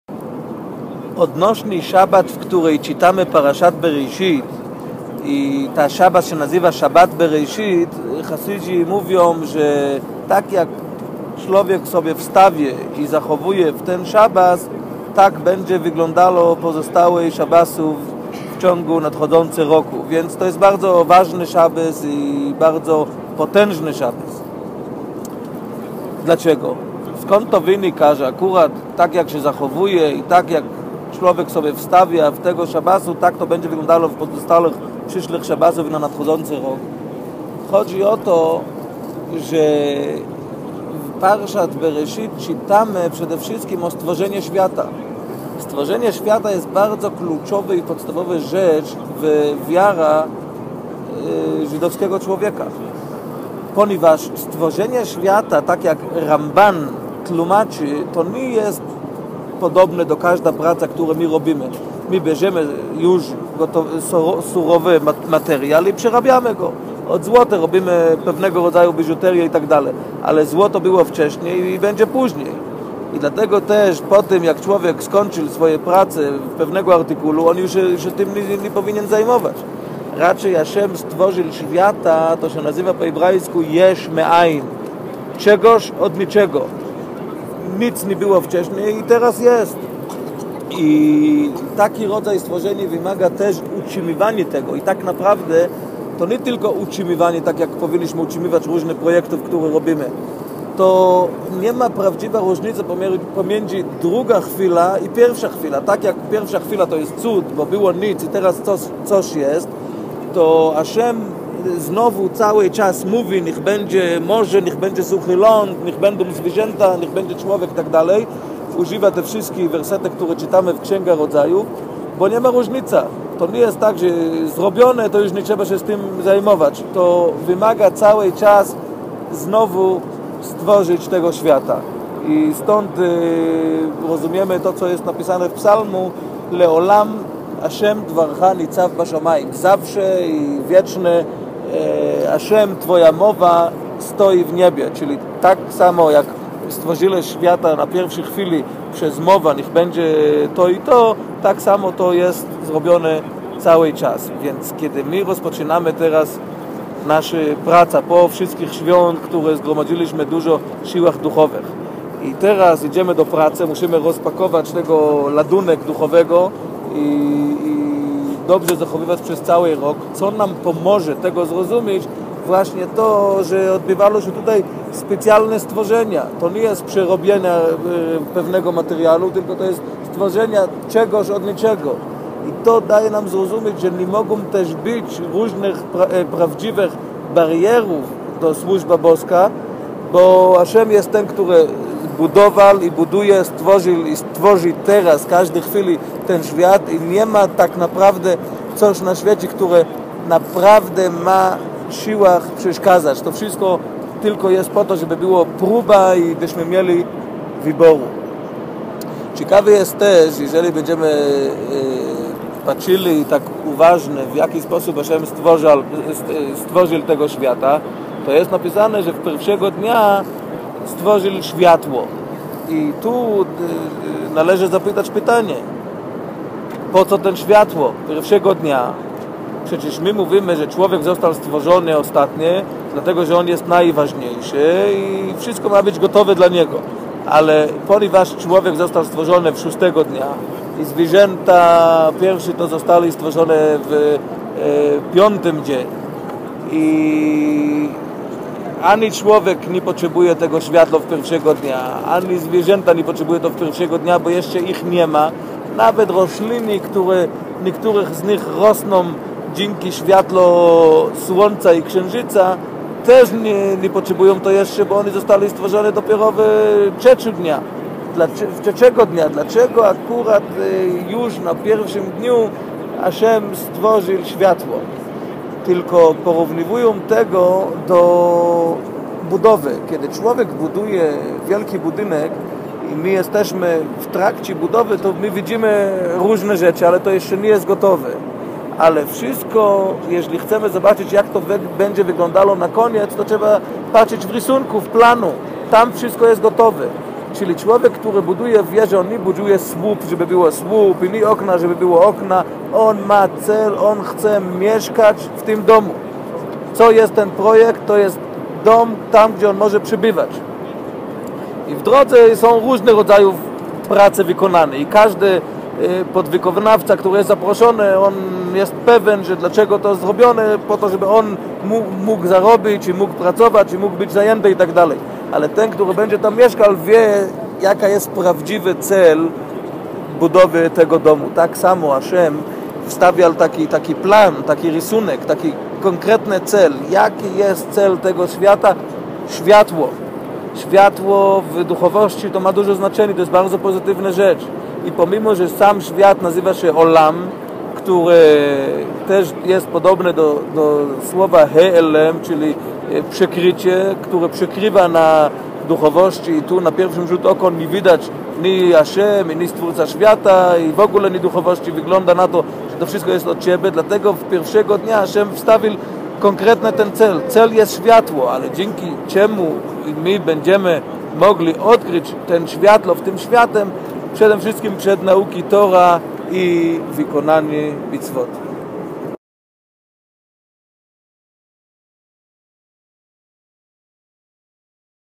Parsza Tory Bereszit
Komentarz Rabina Szaloma Ber Stamblera do Parszy Tory Bereszit - Księga Tory Bereszit - בראשית (Księga Rodzaju), 1:1 – 6:8